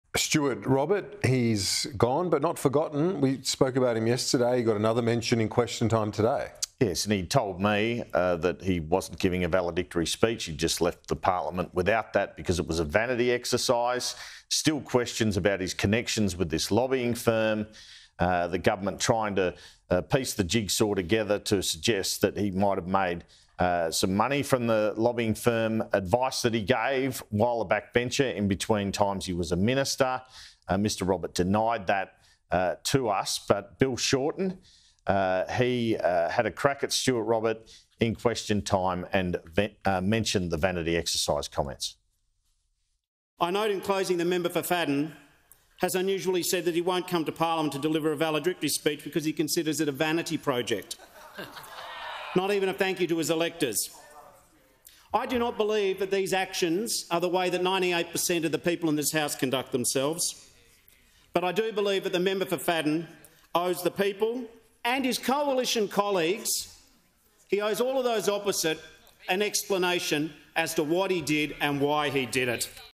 'Vanity exercise': Stuart Robert chooses not to deliver valedictory speech in parliament
Bill Shorten has “had a crack” at Stuart Robert during Question Time over his decision not to deliver a valedictory speech in parliament as he considers it a “vanity exercise”, says Sky News Political Editor Clennell.

“He told me that he wasn’t giving a valedictory speech, he just left the parliament without that because it was a vanity exercise,” Mr Clennell said.